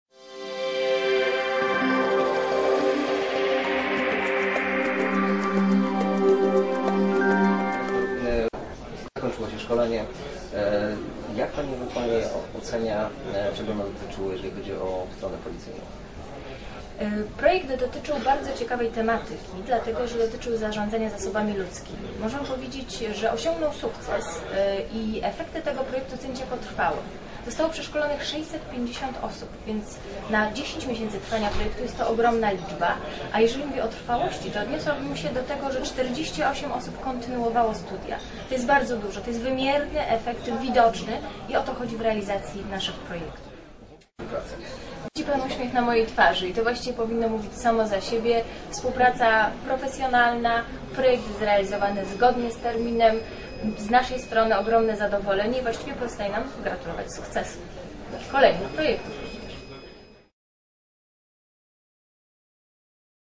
Wypowiedź Karoliny Zelent-Śmigrodzkiej o projekcie zrealizowanym przez KSP
- Portal Edukacyjny Komendy Stołecznej Policji
Wypowiedź p. Karoliny Zelent-Śmigrodzkiej z Ministerstwa Spraw Zagranicznych podczas konferencji "Mechanizmy wspierające polskie instytucje" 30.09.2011 r.